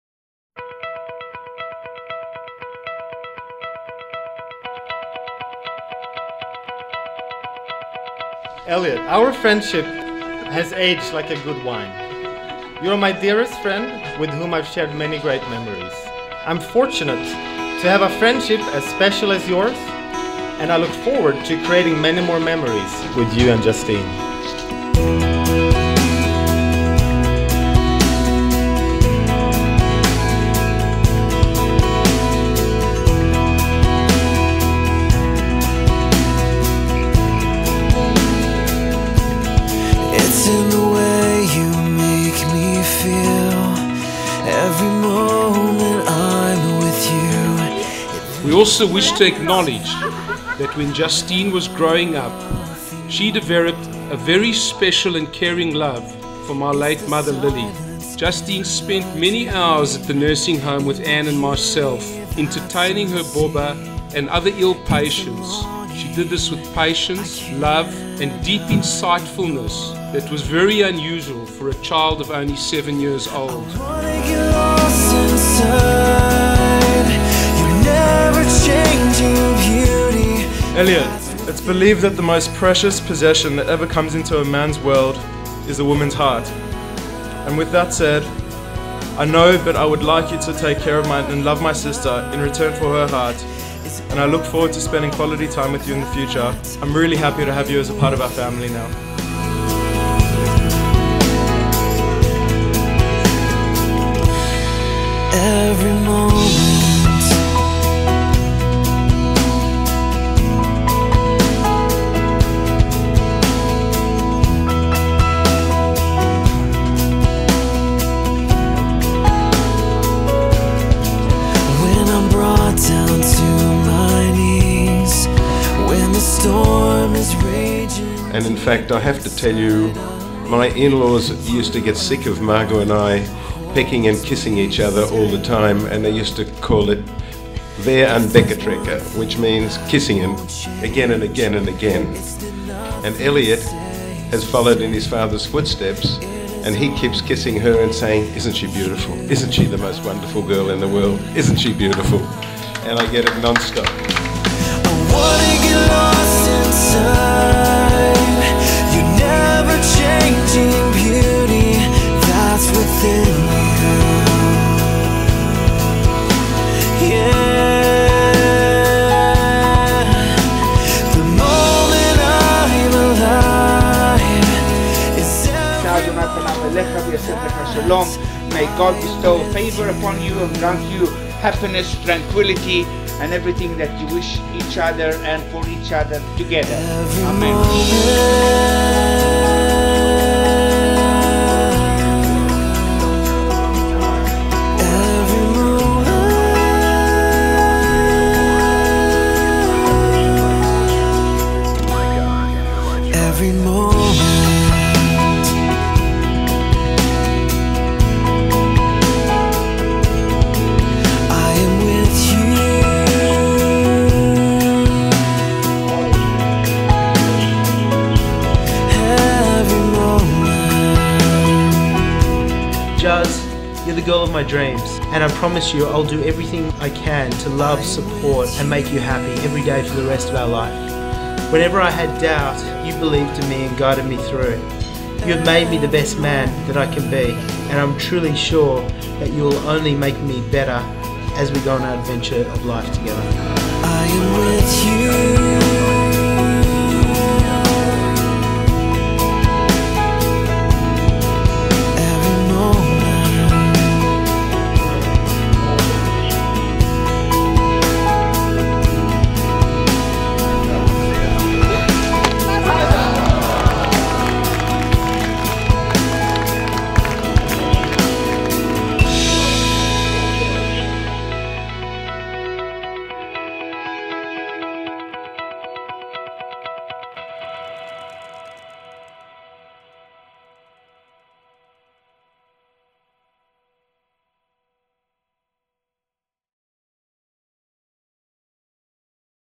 Justine & Elliott, NSW
A beautiful, big white Jewish wedding; by Papercranes